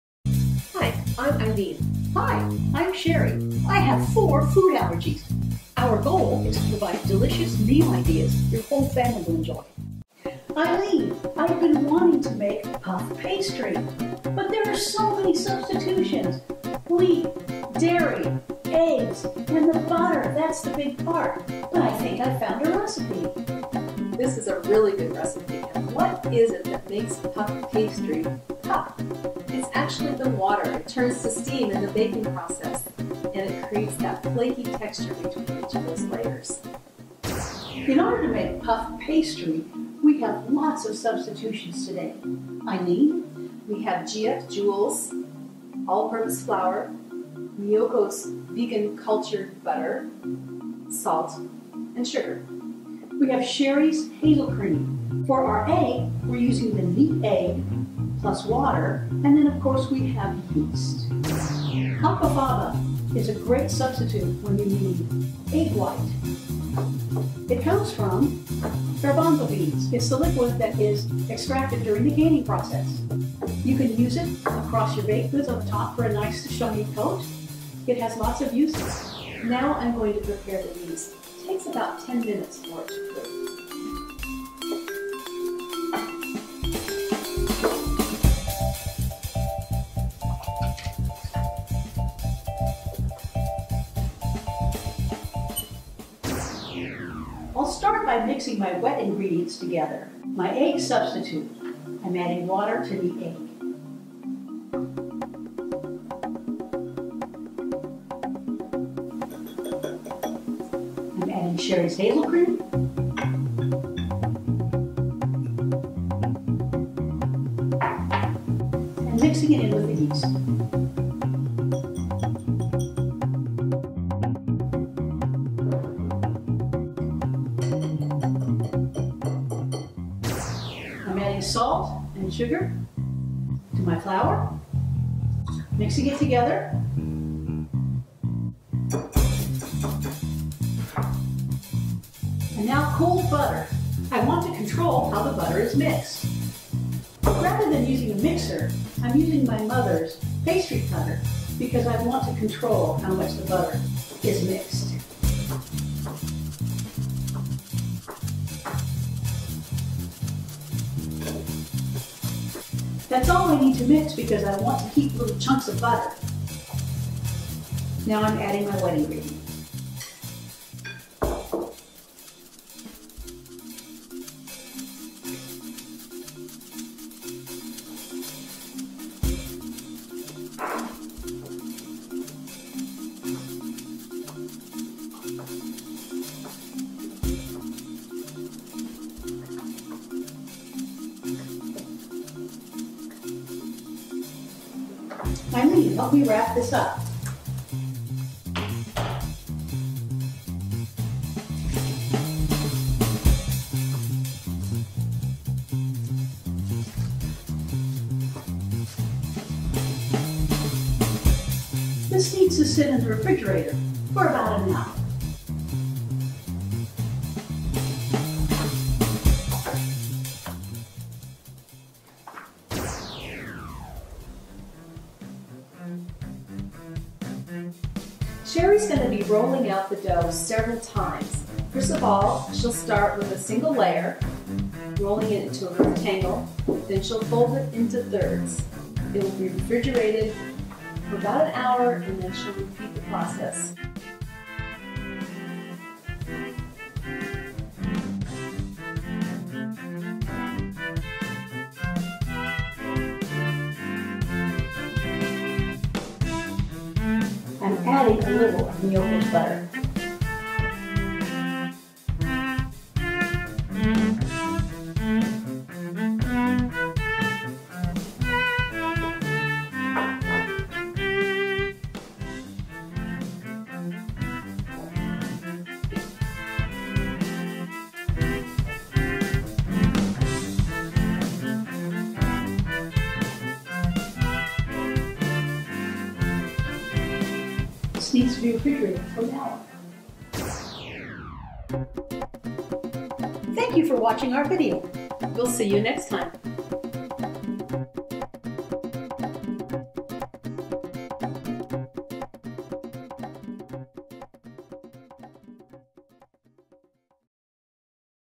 Puffed Pastry Dough
Cheri and Eileen make delicious Puffed Pastry Dough that can be the base for a variety of sweet and savory meal ideas. As usual, this dish is Gluten Free, Egg free and now Dairy Free, thanks to Cheri's HazelCream, a new non dairy milk substitute especially designed for cooking.